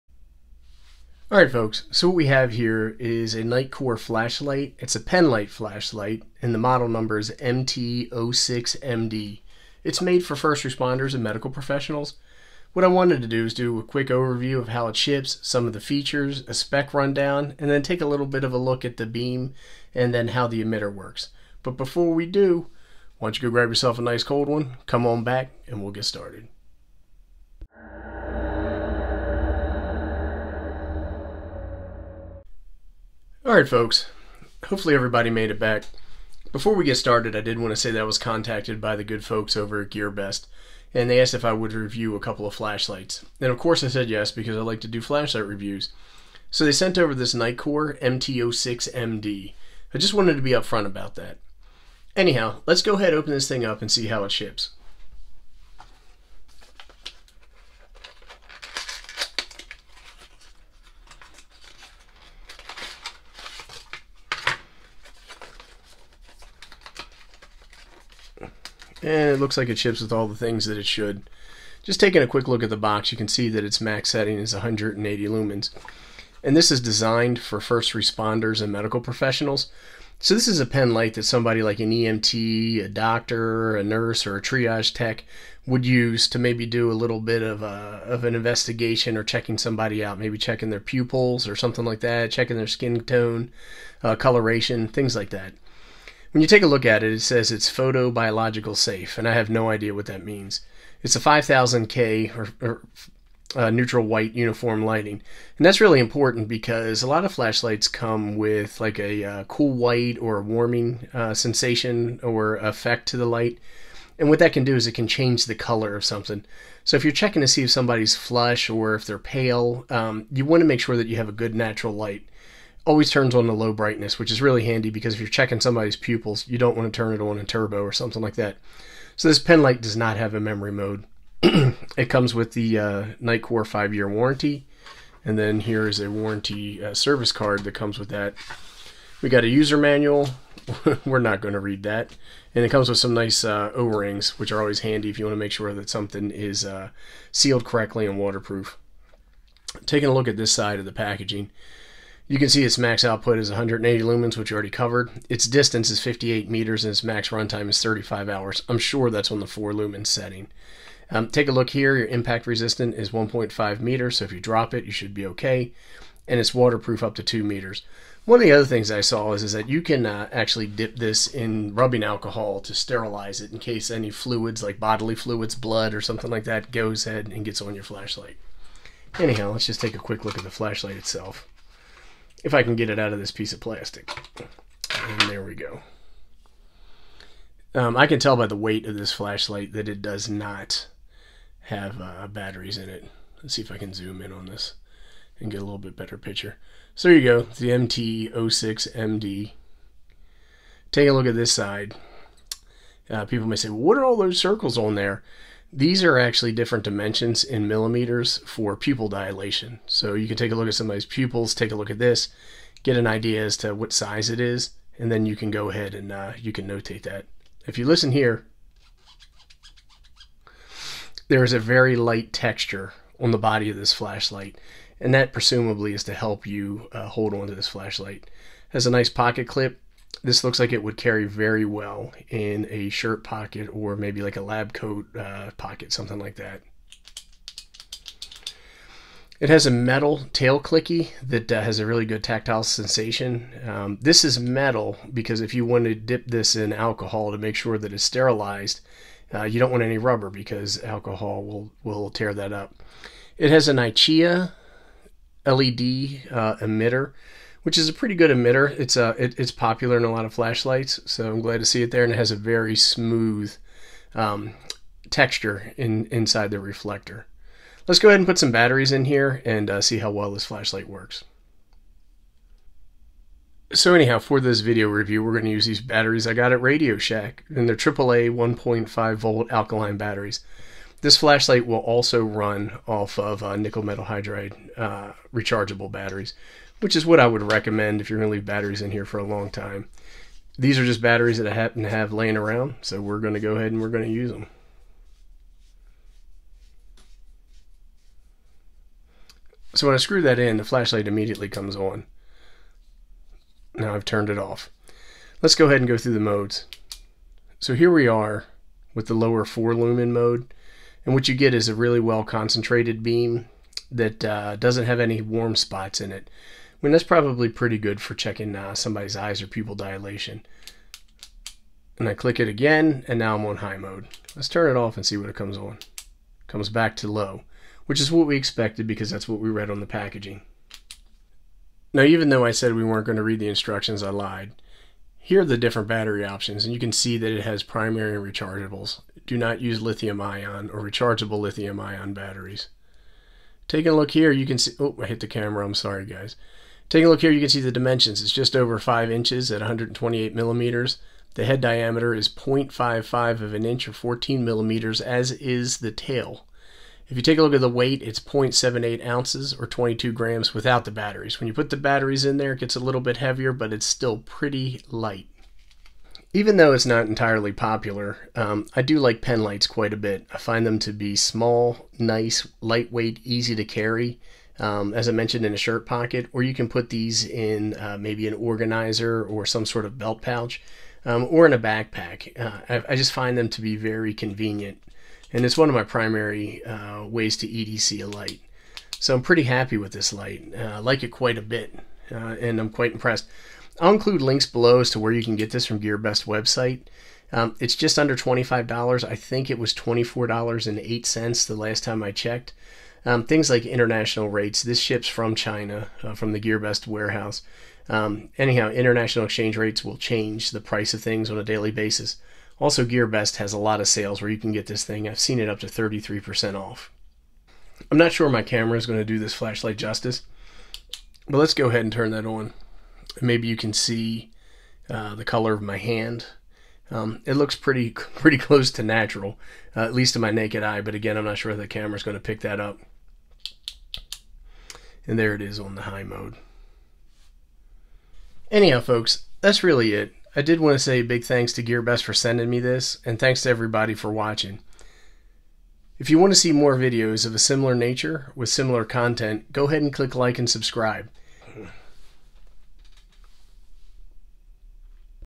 Nitecore MT06MD Penlight Flashlight - GearBest - TheSmokinApe
This penlight was sent to me free of charge from Gearbest.com in exchange for this video review.

The Nitecore MT06MD is an update to the MT06 penlight designed for medical personnel and first responders. Equipped with a Nichia 219B LED, this compact penlight offers a maximum output of 180 lumens with a CRI rating  less than 90 for accurate color rendering to improve injury diagnosis. Safe for use on the eyes, the MT06MD defaults to a 4 lumen 'low mode' and is rated RG0 for safety. This convenient, lightweight penlight weighs less than an ounce before battery, making it an essential tool for doctors, nurses, EMTs, and other medical professionals.

Specifications:
• LED: Nichia 219B
• Output / Runtimes:
--- ○ High: 180 Lumens / 45 min
--- ○ Mid: 45 Lumens / 5 hr 15 min
--- ○ Low: 4 Lumens / 35 hr
• Beam Distance: 63 Yards (58m)
• Beam Intensity: 860cd
• Battery Type: 2x AAA
• Dimensions:
--- ○ Length: 5.04’’ (128mm)
--- ○ Head diameter: 0.55’’ (14mm)
--- ○ Tail diameter: 0.55’’ (14mm)
• Weight: 0.78oz (22g) (without battery)
• Waterproof: IPX8 (2m submersible)
• Impact Resistance: 1.5m

Features:
• Designed for use as a medical inspection light
• Utilizes a Nichia 219B LED (CRI less than 90), max output of 180 lumens
• Photobiologically safe (RG0),color temperature CCT=5000K
• Turns on in “low” by default, convenience and ideal for pupil inspections
• Powered by 2 x AAA batteries, max runtime up to 35 hours
• 3 brightness levels available
• Reverse clicky tail switch provides easy one-handed operation
• High efficiency constant current circuit provides stable and consistent output
• Stainless steel clip
• Aerospace grade aluminium alloy construction
• HAIII hard anodizing

Thanks,
TheSmokinApe